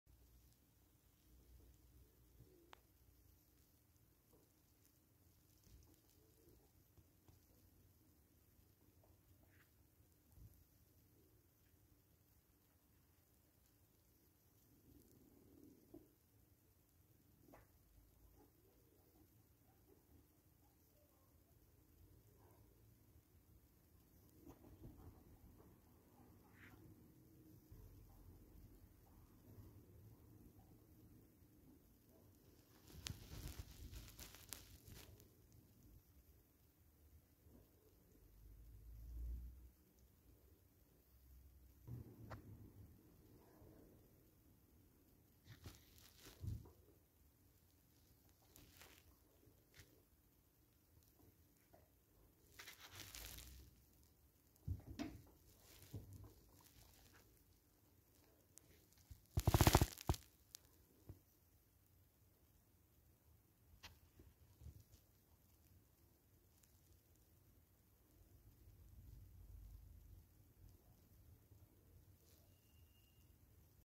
Guinea Pigs Popcorning for their washing
The babies love the washing rack!